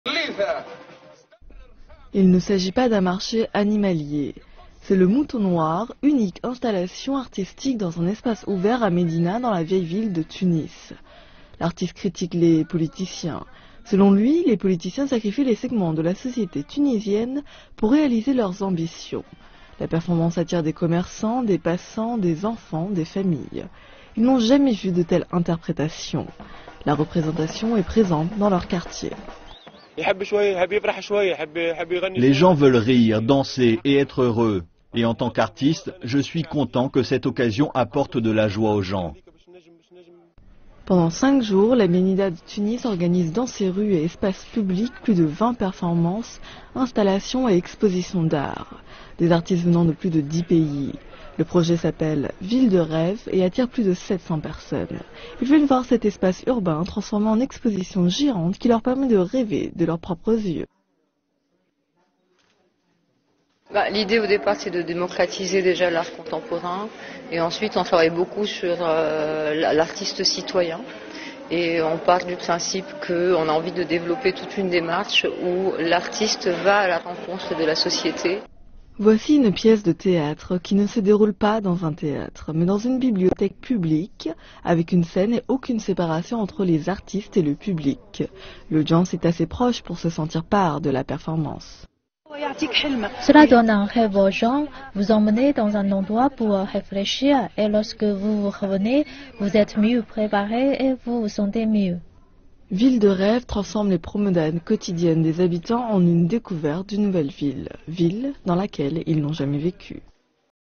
La street art dans les rues de Tunis
La vieille ville de Tunis organise un évènement unique appellé 'Ville de rêve'. Les artistes de 10 pays s'emparent des espaces publics pour mettre en place des installations, des performances et des expositions.